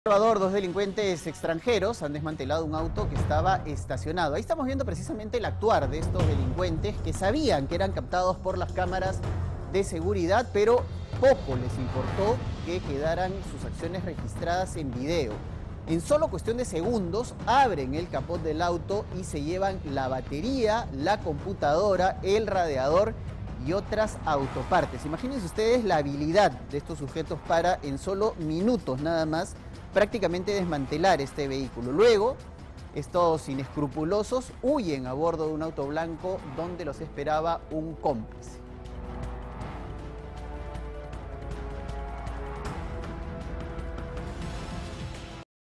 VES: captan a ciudadanos extranjeros desmantelando un vehículo
En Villa El Salvador, dos delincuentes extranjeros desmantelaron un auto que estaba estacionado.

Los ladrones llegaron en un auto de color blanco. Ellos sabían que eran captados por las cámaras de seguridad, pero eso poco les importó, solo se cercioraron que nadie los descubra.

En cuestión de segundos abrieron el capó del auto. Se llevaron, la batería, computadora, radiador y otras autopartes. Los ladrones huyeron en el mismo vehículo blanco donde estaba su tercer cómplice.